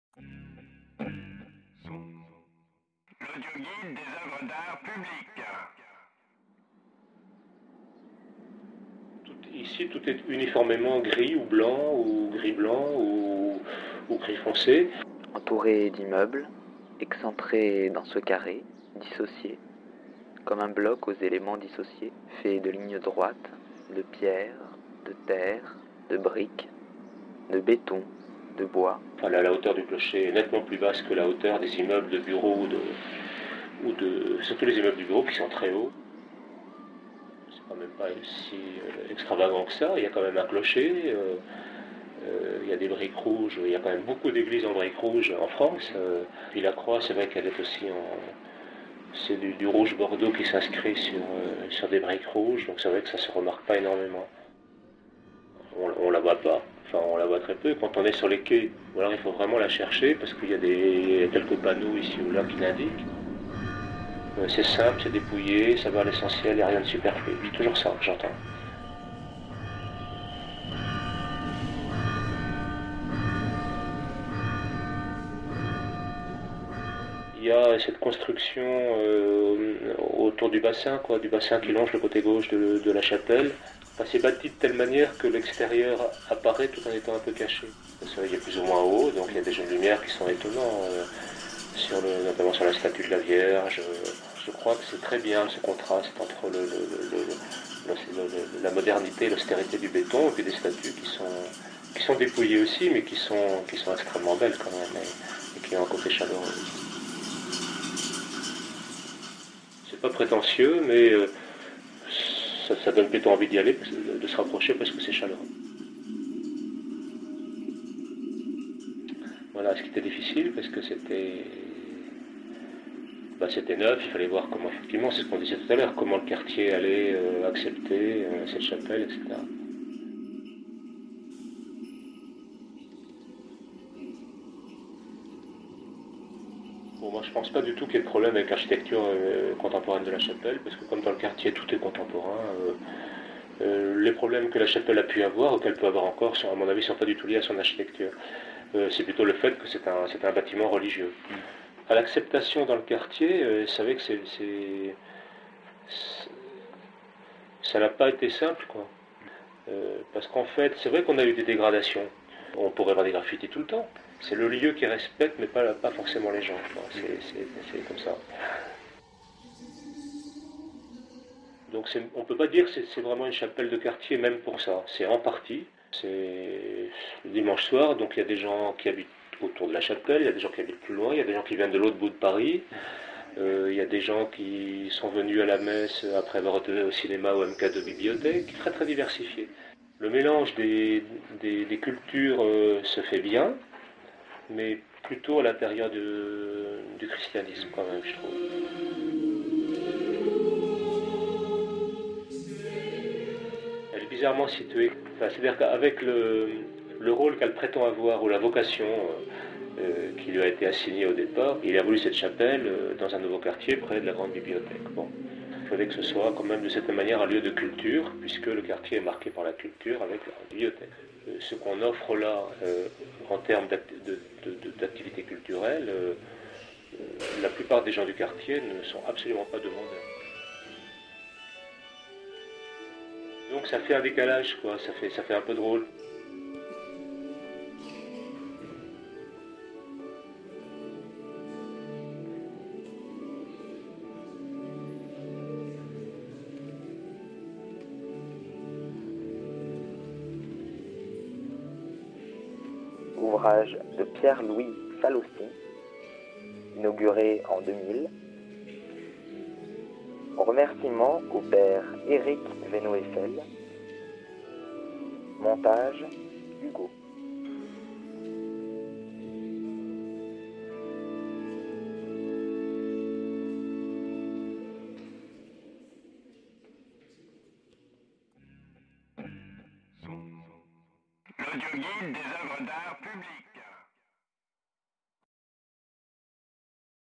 Chapelle Notre-Dame-de-la-Sagesse, Pierre-Louis FALOCI
Notre-Dame-de-la-Sagesse,

Pierre-Louis FALOCI, 2000

Construite en 2000 dans le nouveau quartier de la BNF, la Chapelle Notre-Dame-de-la-Sagesse est intégrée (excentrée) au square James Joyce. Elle se démarque par sa brique rouge contrastant et dialoguant à la fois avec les bâtiments modernes, HLM et bureau, qui encadrent le square, ainsi qu'avec la végétation environnante.

Dessiné par Pierre-Louis Faloci, la chapelle de type sobre et discrète passe presque inaperçus auprès des passants.
Pensé comme lieu de recueillement pour les habitants et les employés du quartier, la rencontre se révèle tout à la fois riche et difficile à l'image d'un intérieur en béton dépouillé et chaleureux filtrant la lumière extérieur à la fois ouvert et isolé du monde.
Dans une démarche architectural proche de celle de Tadao Ando, influencé par la chapelle Notre-Dame-de-Ronchamp de Le Corbusier, Notre Dame de la Sagesse, bâtiment moderne, met en question la place de l'église dans la ville du XXIeme siècle.

Hugo DE OLIVEIRA